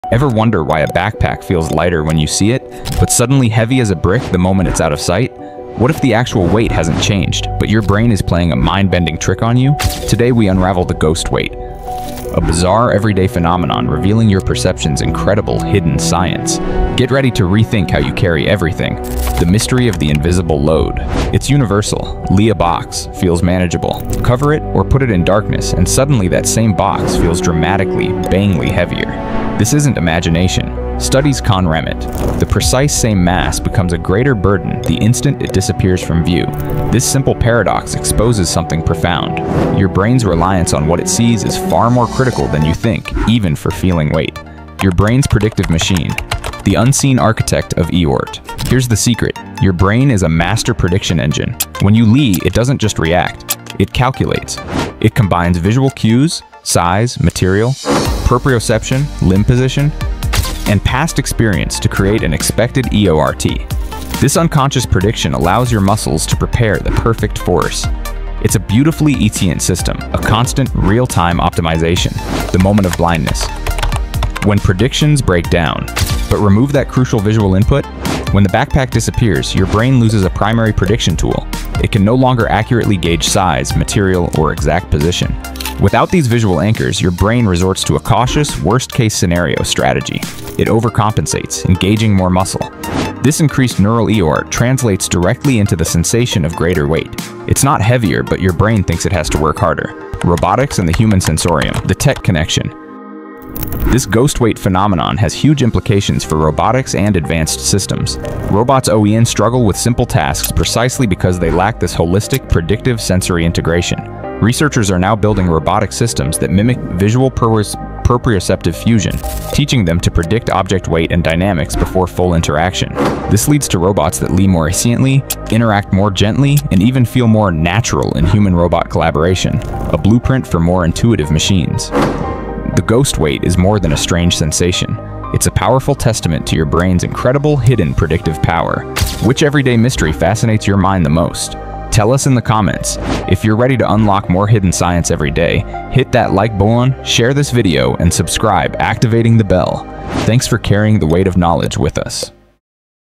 The Ghost Weight Illusion: Why Things Feel Heavier When You Can’t See Them
Ever wonder why a backpack feels heavier the moment it's out of sight? What if the weight hasn't changed, but your brain is playing a mind-bending trick on you? Welcome to the "Ghost" Weight phenomenon—a bizarre, everyday experience revealing your brain's incredible, hidden science.

This video dives deep into the mystery of why your brain's reliance on what it sees is far more critical than you think. We'll explore how your brain works as a master prediction engine, how this system breaks down without visual cues, and what these insights mean for the future of robotics and human-machine collaboration.

Video Chapters:
0:00 The Invisible Burden
0:15 The Mystery of the Invisible Load
0:50 Your Brain's Predictive Machine
1:30 When Predictions Break Down
2:15 The Tech Connection: Robotics & The Human Sensorium and The Learning? Your Brain's Hidden Power!